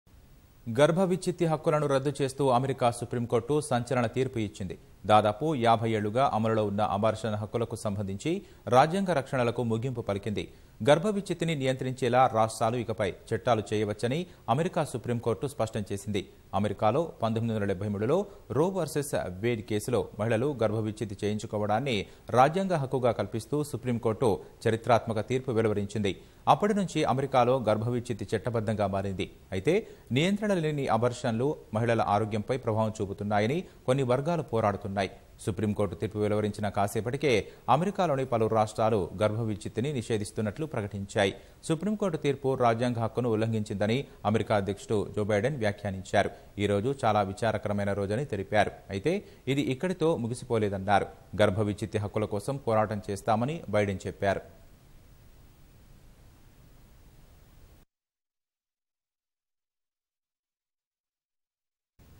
US Supreme Court Kills Abortion Rights
EtvTelanganaగర్భవిచ్చిత్తి హక్కులను రద్దు చేస్తూ అమెరికా సుప్రీంకోర్టు సంచలన తీర్పు ఇచ్చింది.
దాదాపు యాభై ఏళ్లుగా అమలులో ఉన్న అబార్షన్ హక్కులకు సంబంధించి... రాజ్యాంగ రక్షణలకు ముగింపు పలికింది. గర్భవిచ్చిత్తిని నియంత్రించేలా. రాష్ట్రాలు ఇకపై చట్టాలు, చేయవచ్చని అమెరికా సుప్రీంకోర్టు స్పష్టం చేసింది. అమెరికాలో. 1973లో ‘రో వర్సెస్ వేడ్’కేసులో...మహిళలు గర్భవిచ్చిత్తి చేయించుకోవడాన్ని రాజ్యాంగ హక్కుగా కల్పిస్తూ సుప్రీంకోర్టు చరిత్రాత్మక తీర్పు వెలువరించింది. అప్పటి నుంచి అమెరికాలో గర్భవిచ్చిత్తి చట్టబద్ధంగా మారింది. ఐతే. నియంత్రణ లేని అబార్షన్లు, మహిళల ఆరోగ్యంపై ప్రభావం చూపుతున్నాయని..కొన్ని వర్గాలు పోరాడుతున్నాయి. సుప్రీంకోర్టు తీర్పు వెలువరించిన కాసేపటికే అమెరికాలోని పలు రాష్ట్రాలు. గర్భవిచ్చిత్తిని నిషేధిస్తున్నట్లు ప్రకటించాయి. సుప్రీంకోర్టు తీర్పు రాజ్యాంగ హక్కును ఉల్లంఘించిందని అమెరికా అధ్యక్షుడు జోబైడెన్ వ్యాఖ్యానించారు. ఈ రోజు చాలా విచారకరమైన రోజని తెలిపారు. అయితే ఇది ఇక్కడితో ముగిసిపోలేదన్నారు. గర్భవిచ్చిత్తి హక్కుల కోసం పోరాటం చేస్తామని... బైడెన్ చెప్పారు.